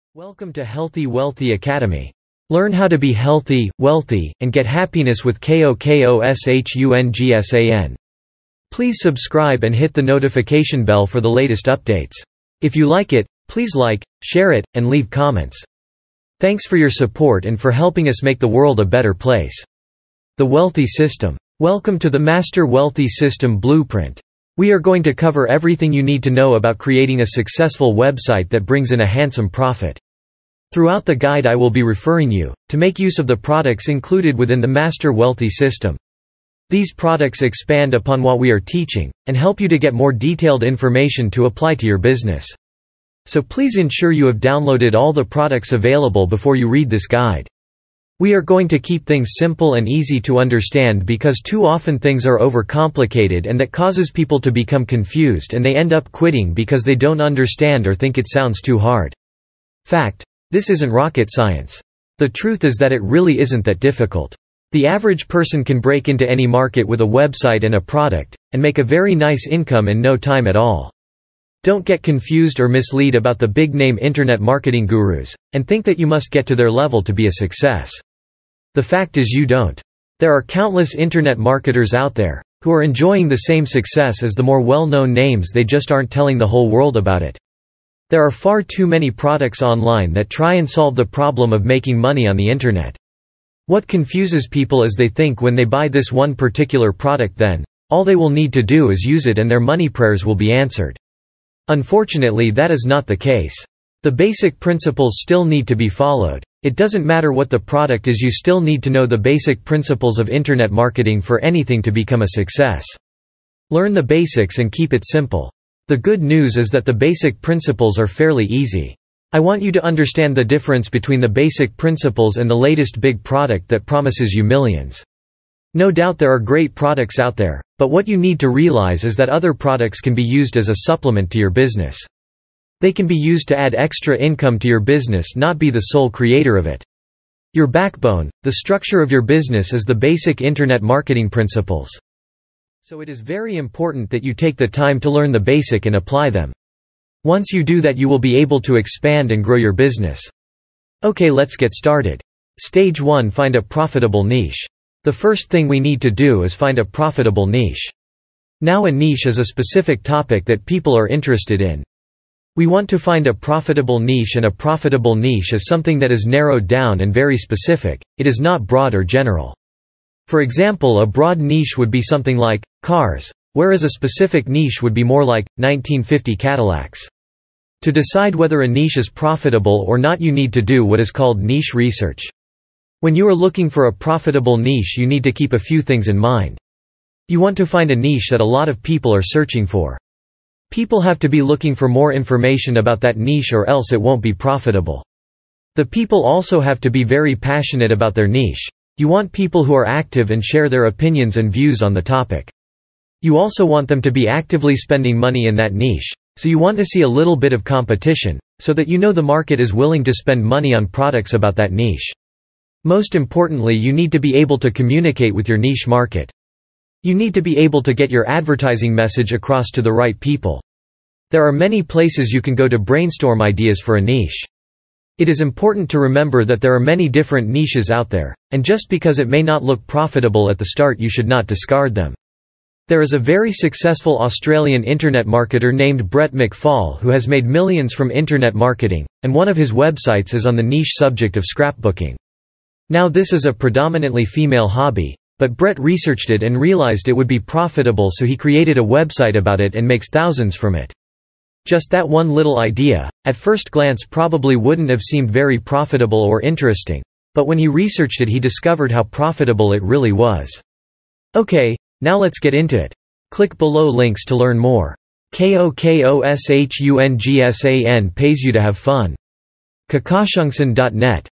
How To Create a Successful Website With A Profitable Niche
Learn How To Create a Successful Website With A Profitable Niche. We are going to cover everything you need to know about creating a successful website that brings in a handsome profit.

TIMESTAMPS
00:00 Intro​
00:20 Stage 1: Find A Profitable Niche
04:00 Final
Stage 1.1: How To Discover The Needs & Problems Of Your Niche
Stage 2
Stage 2.1: Rapidly Create Products
Stage 2.2: Create A High Converting Sales Page
Stage 2.3: Send A Bucket Load Of Traffic To Your Website
Stage 3
Stage 3.1: Backend Profits Rinse & Repeat Conclusion
【  Full Course 】  ► The Wealthy System with MRR:

💗KOKOSHUNGSAN® Pays You To Have Fun! The Fun Never Stops!